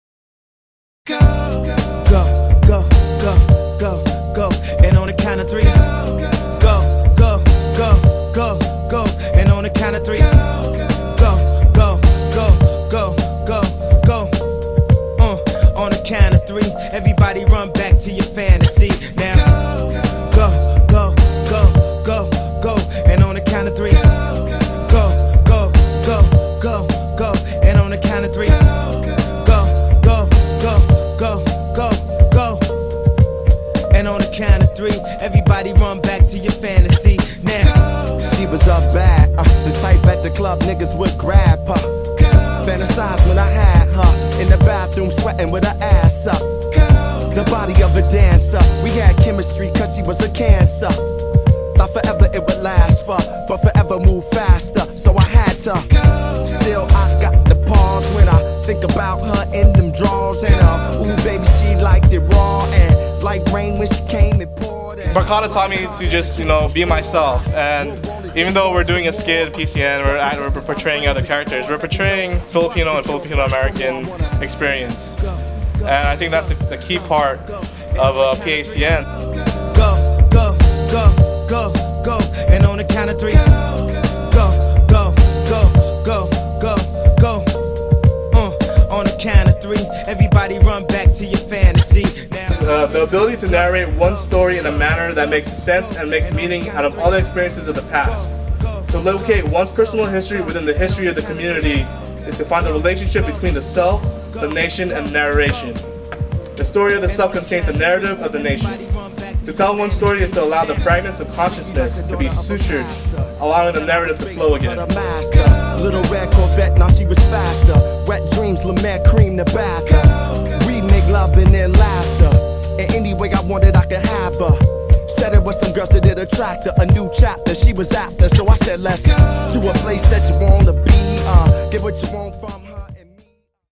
Hawak Kamay Sneak Peak
Cal Poly Pomona Barkada's 22nd annual PACN is this Friday, May 4th at the Haugh Performing Arts Center at Citrus College. Doors open at 5:45PM, and the show begins at 6:45PM. We hope to see you there!